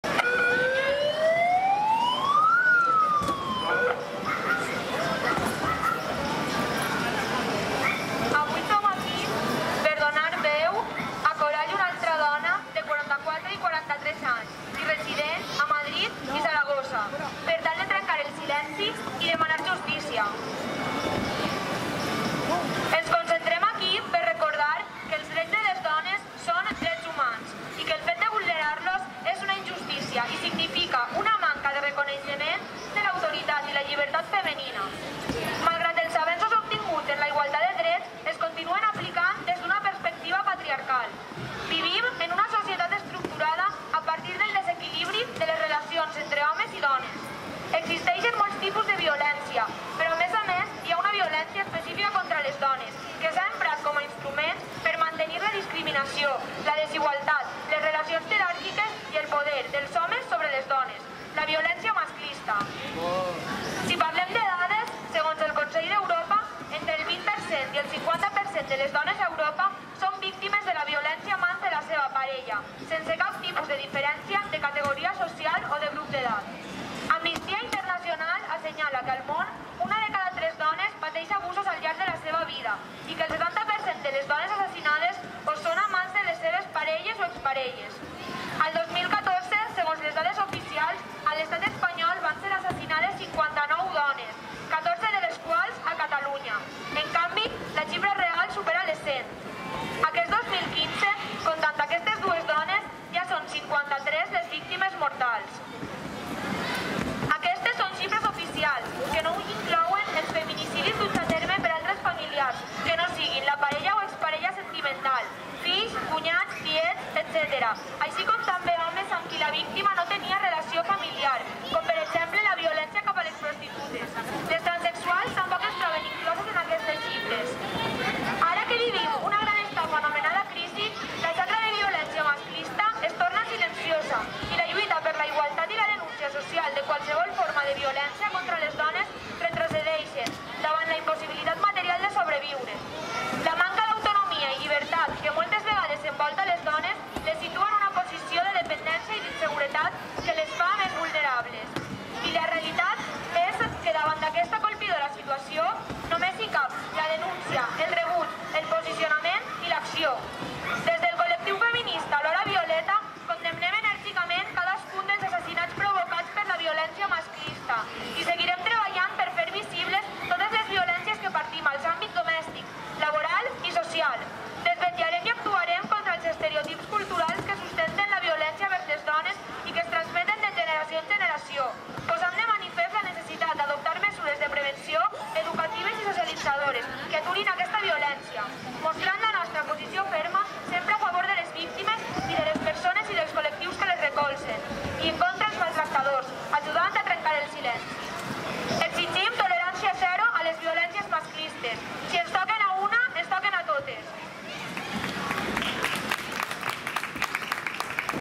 Concentració contra la violència de masclista a Reus. 17 de desembre de 2015
Dijous 17 de desembre del 2015. Aquest cap de setmana han mort assassinades dues dones, una a Alcobendas (Madrid) i l'altra a Saragossa. El terrorismemasclista no s'atura.
Concentració organitzada pel col·lectiu feminista "L'Hora Violeta" contra la violència de envers les dones a la plaça Mercadal de Reus. Aquesta concentració està emmarcada dins la campanya, NoMorimEnsMaten, promoguda per diferents col·lectius feministes dels Països Catalans i, a Reus, es porta a terme des del gener del 2015.